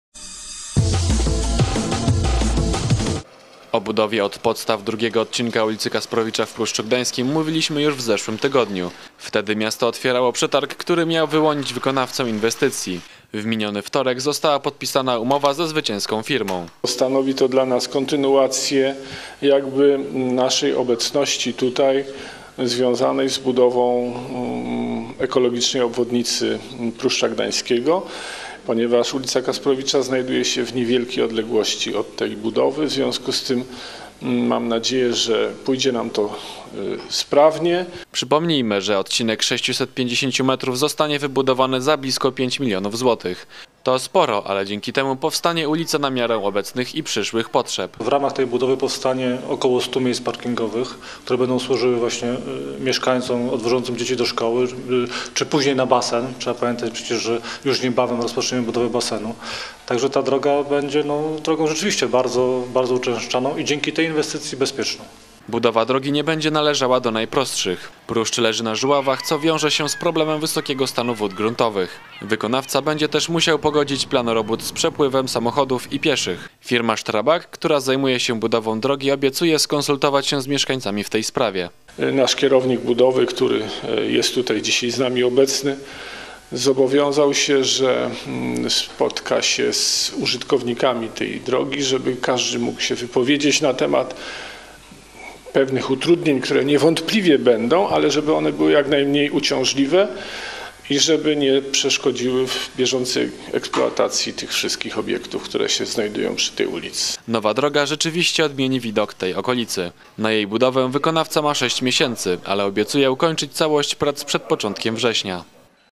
ul. Kasprowicza ma już wykonawcę
Wszystko idzie zgodnie z planem. Do końca marca miał zostać wyłoniony wykonawca na budowę ul. Kasprowicza w Pruszczu Gdańskim. Przetarg na budowę wygrała firma Strabag